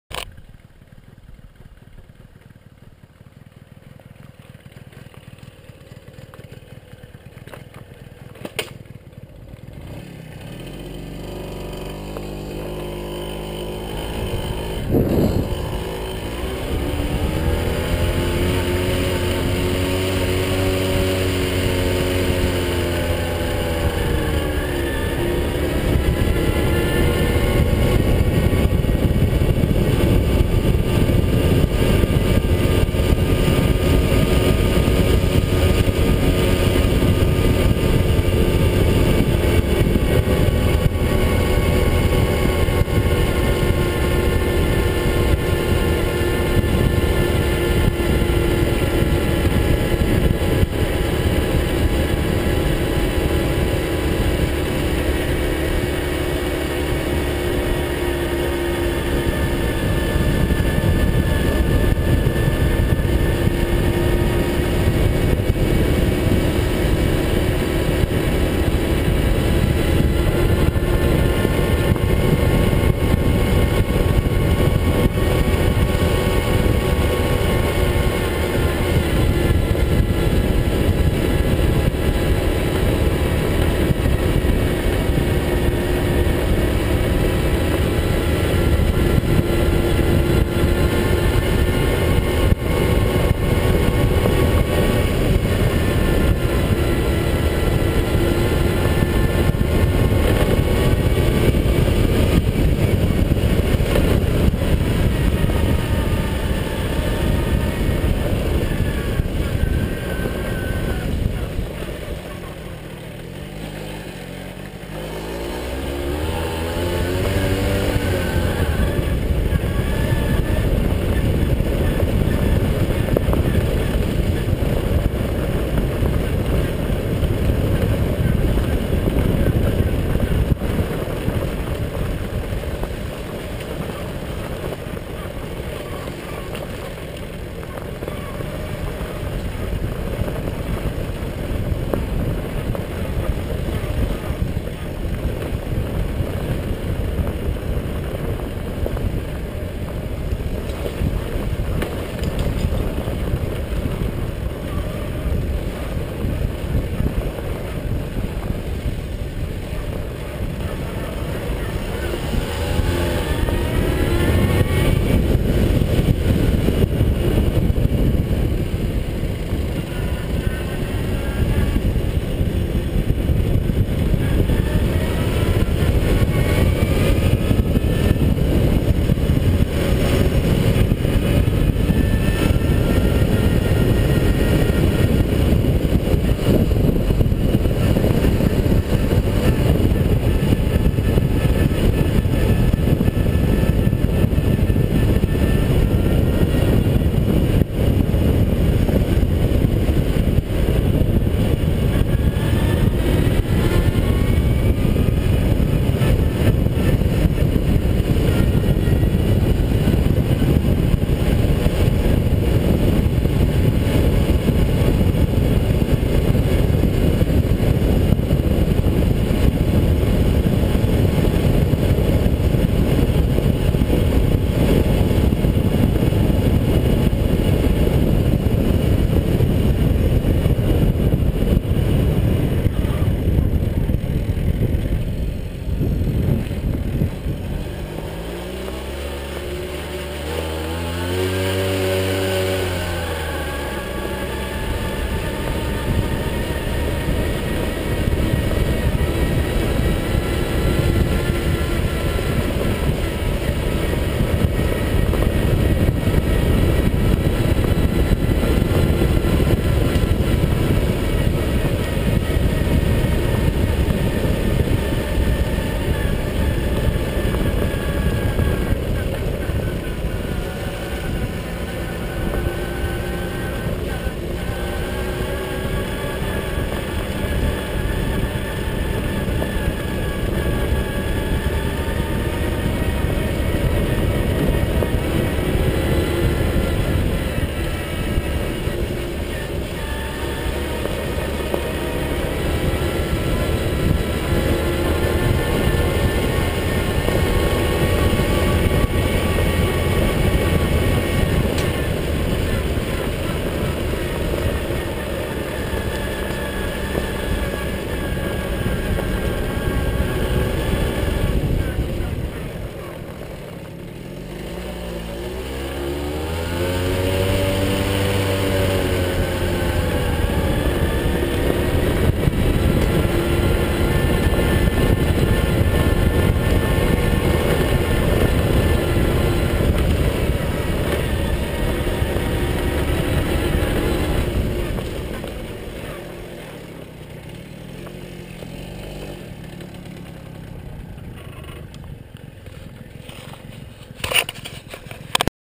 Tomos A3MS 1982 Test drive
Went on a test run with my 1982 Tomos
Shot with: GoPro HD Hero